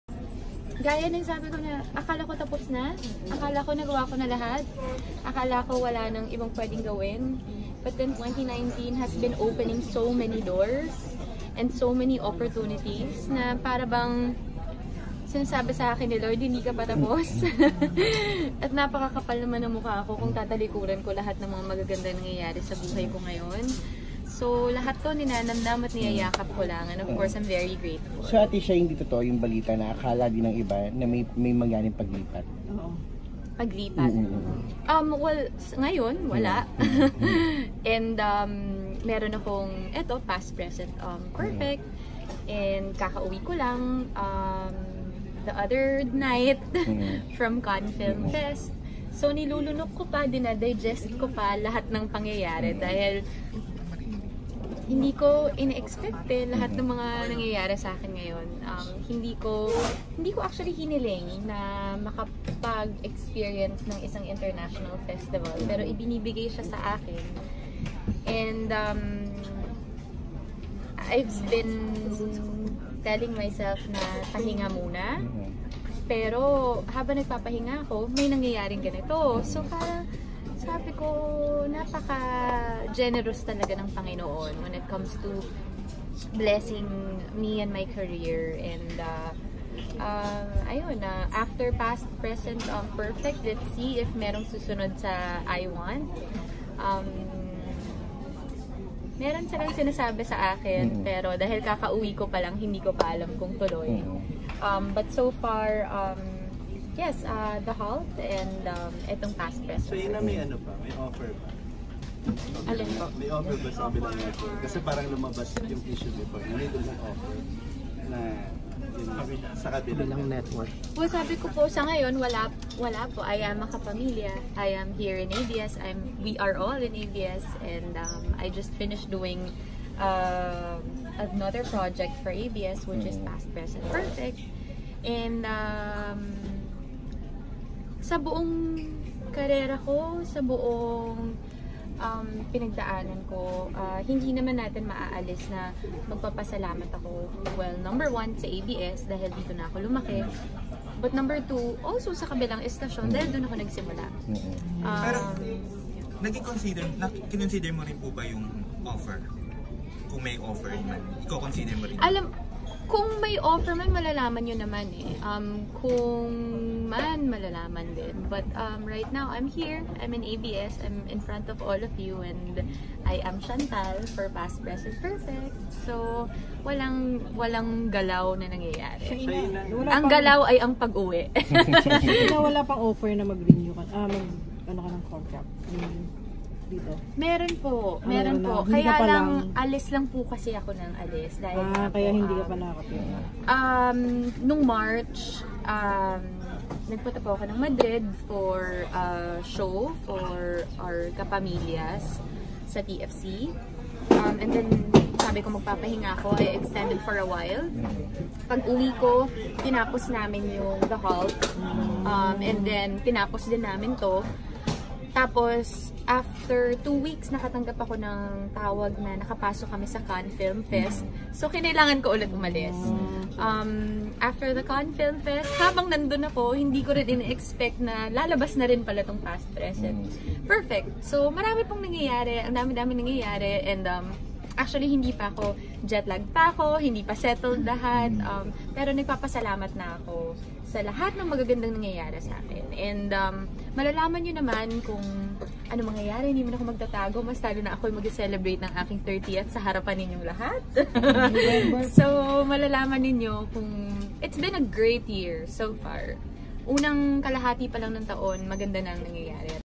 Shaina Magdayao, denies network transfer rumors
Shaina is also part of upcoming series in iWant “Past, Present, Perfect” together with Vin Abrenica and Loisa Andalio